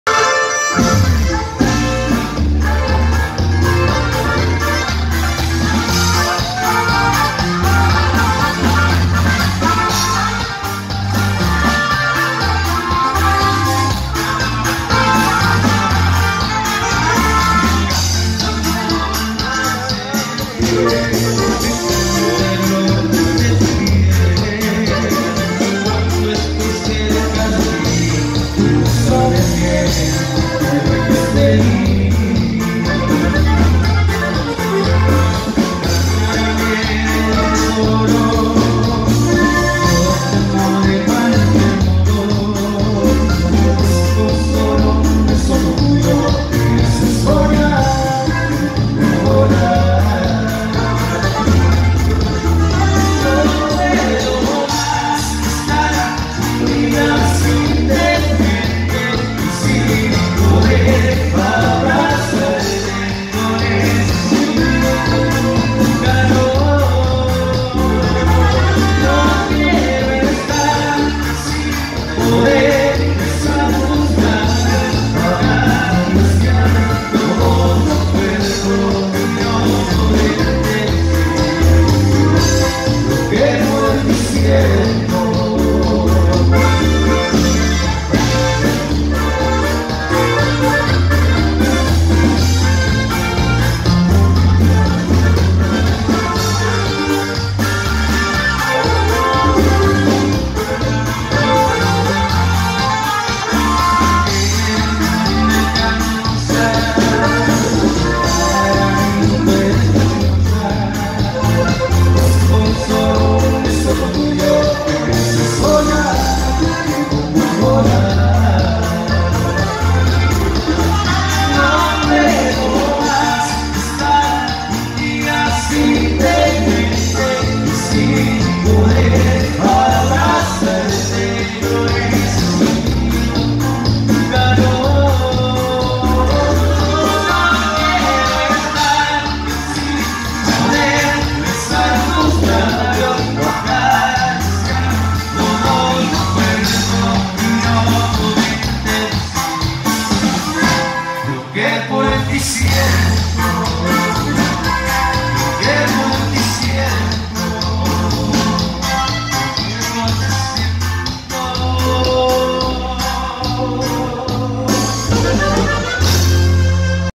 Bailando Corridas en San Pedro Coyame Con Pacto Norteño.
Contrataciones de Pactó Norteño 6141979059.